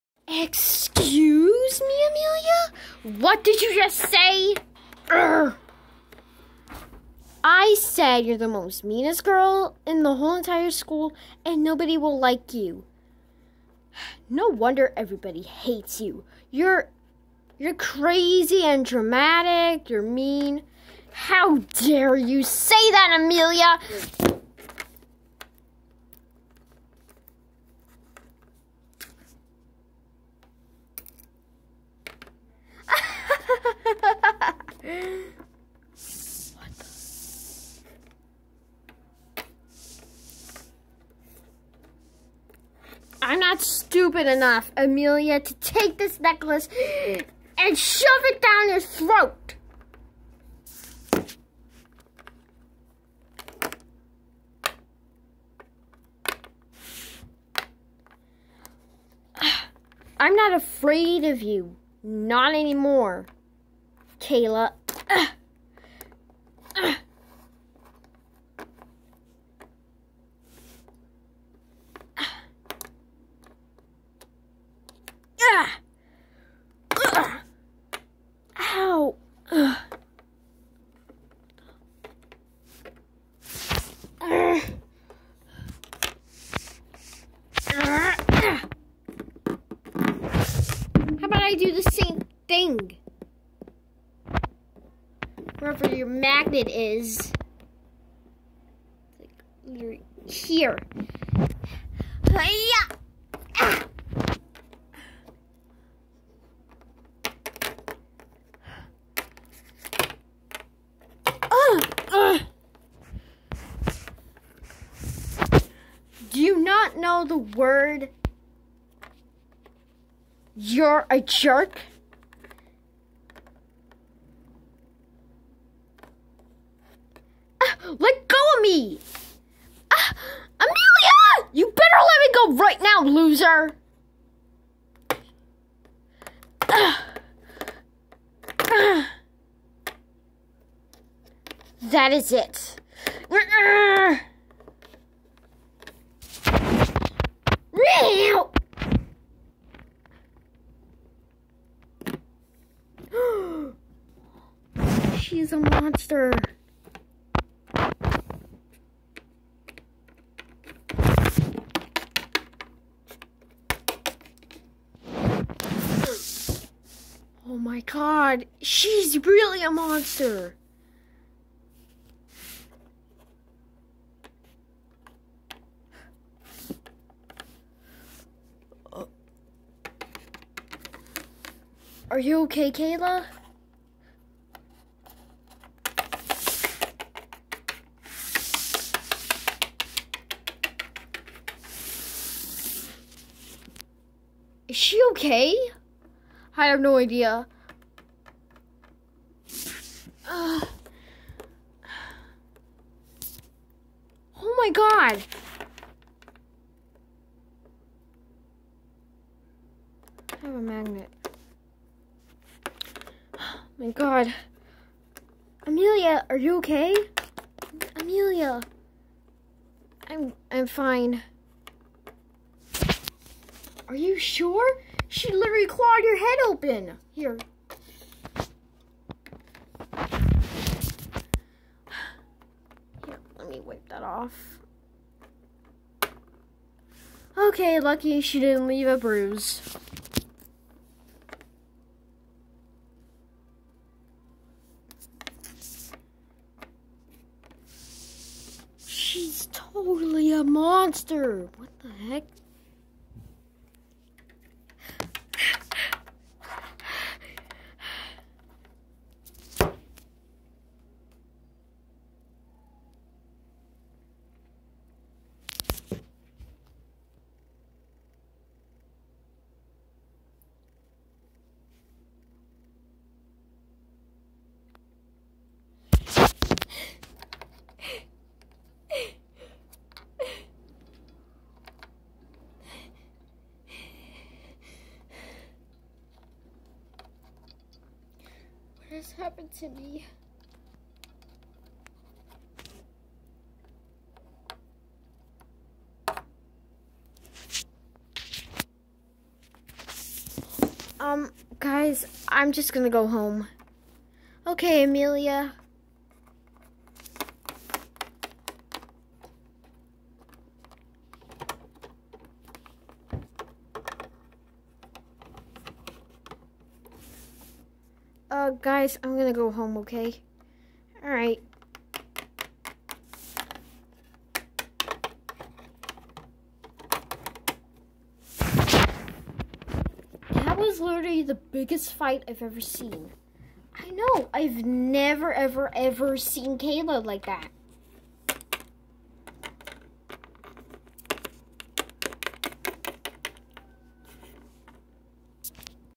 lps Highschool true love Episode 4
Amelia and Kayla get into a Fight and Kayla starts to make the biggest mistake of her life popularity and reputation